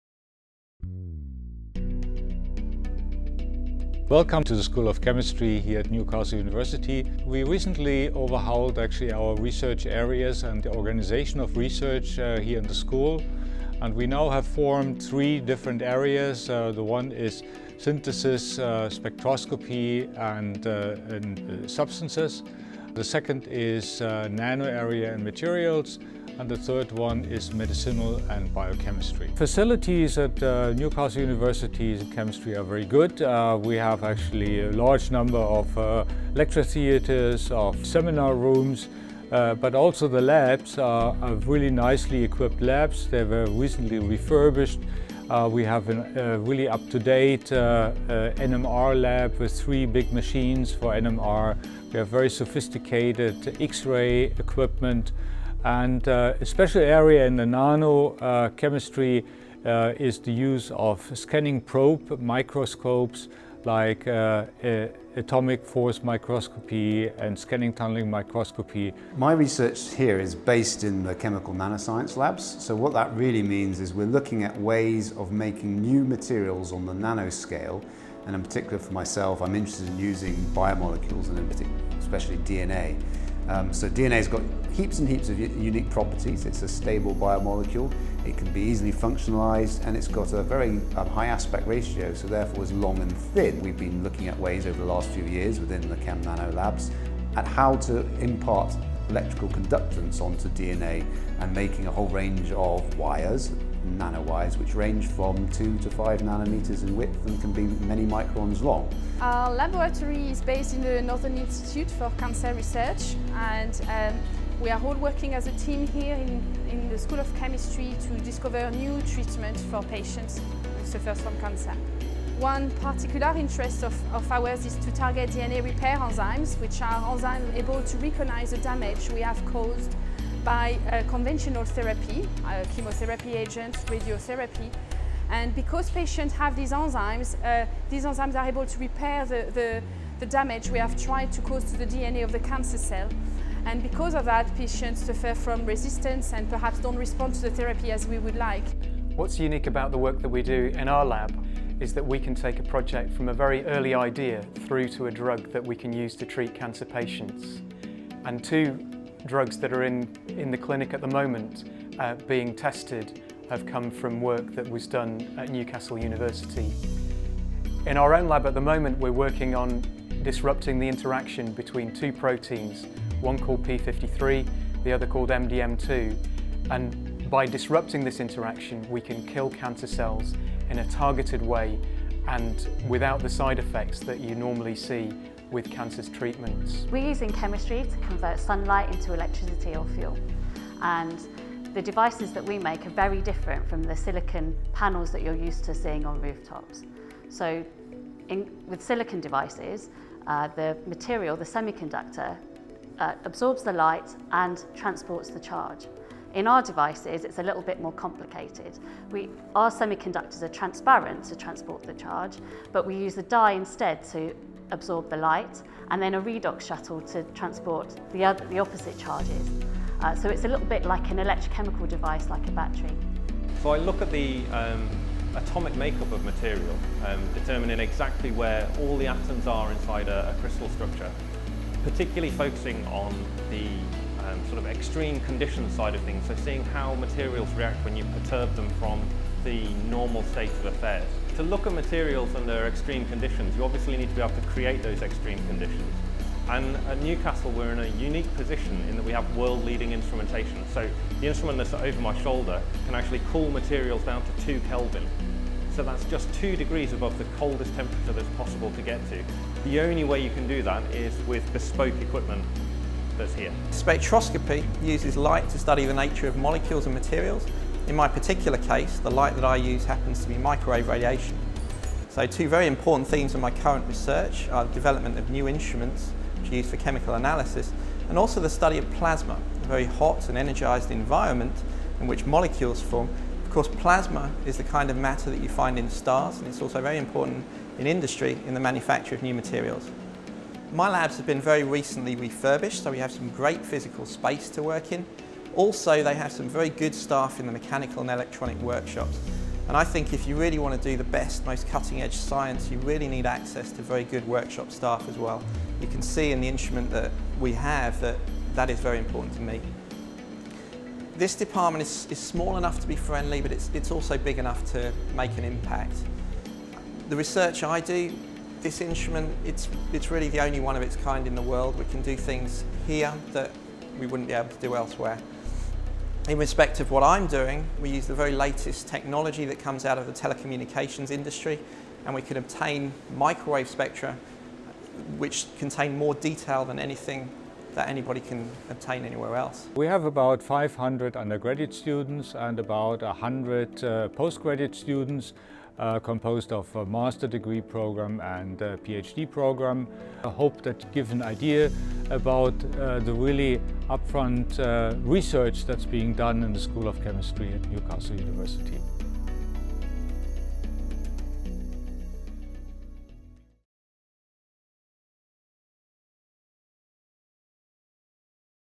Chemistry research at Newcastle University
An overview of research and facilities in the School of Chemistry at Newcastle University